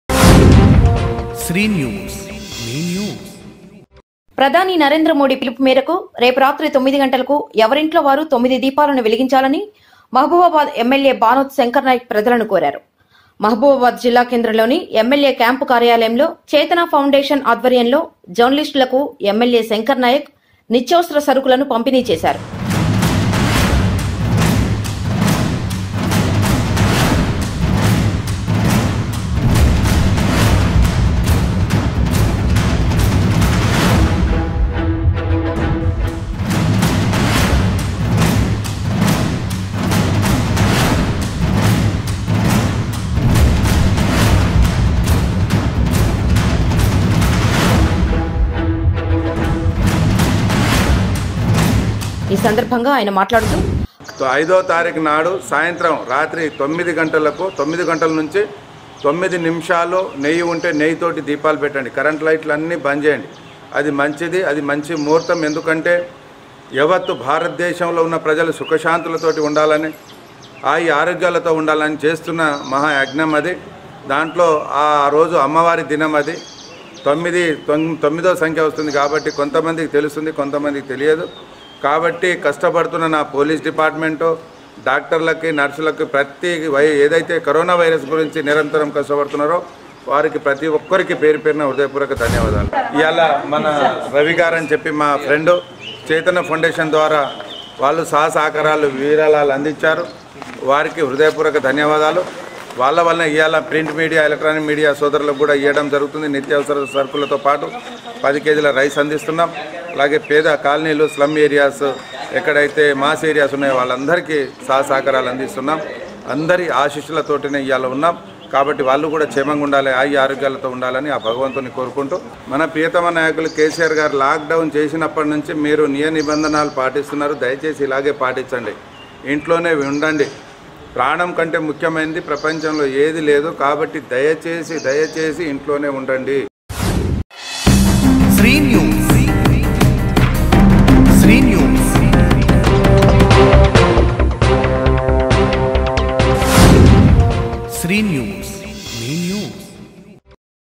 మహబూబాబాద్ లో జర్నలిస్టులకు ఎమ్మెల్యే శంకర్ నాయక్ నిత్యావసర సరుకులను పంపిణీ //SreeNews//Mhbd//
*మహబూబాబాద్ జిల్లా కేంద్రంలోని ఎమ్మెల్యే క్యాంపు కార్యాలయంలో చేతన ఫౌండేషన్ (U S )  ఆధ్వర్యంలో జర్నలిస్టులకు ఎమ్మెల్యే శంకర్ నాయక్  నిత్యావసర సరుకులను పంపిణీ చేశారు*